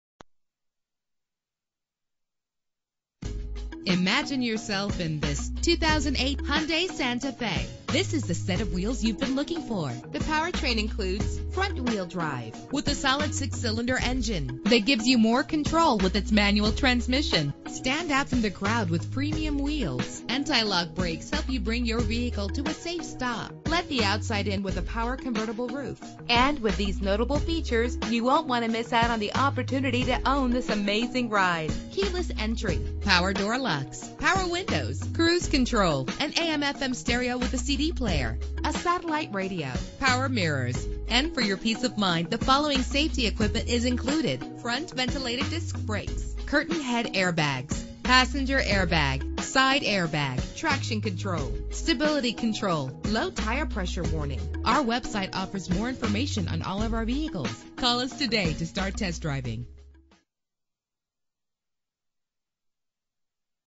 2008 Hyundai Santa Fe - Vancouver WA
Year: 2008
Make: Hyundai
Model: Santa Fe
Trim: GLS
Engine: 2.7 liter 6 cylinder 24 valve
Transmission: 5-Speed Manual
Color: Black
Mileage: 61598
Address:
1200 NE 95th Street